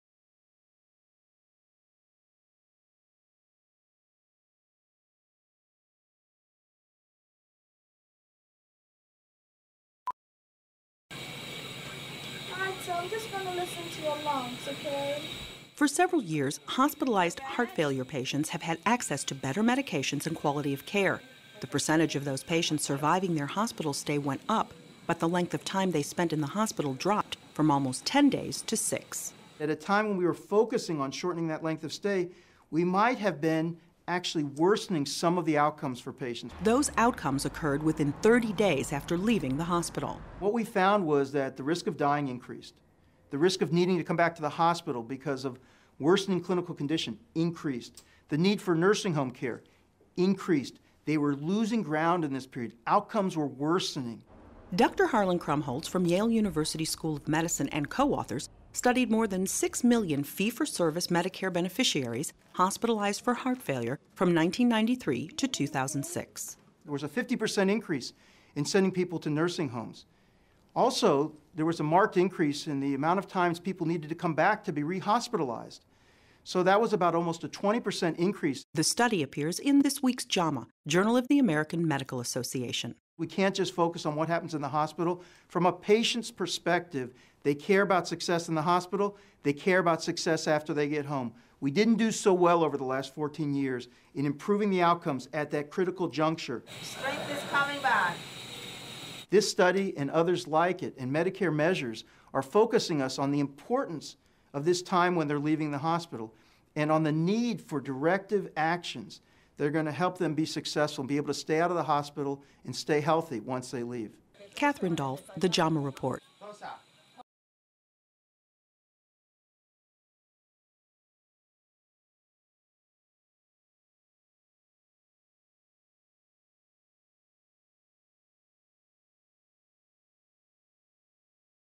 WORSENING OUTCOMES FOR HEART FAILURE PATIENTS AFTER SHORTER AND MORE SUCCESSFUL HOSPITAL STAYS
Over the last decade heart failure patients are experiencing shorter and more successful hospital stays. What happens when they leave the hospital especially in that crucial, 30 day period after discharge had been unclear. A new study shows the risk of being admitted to a nursing home, re-admitted to the hospital and dying in the month following hospital discharge is rising. Catherine Dolf has more in this week's JAMA Report.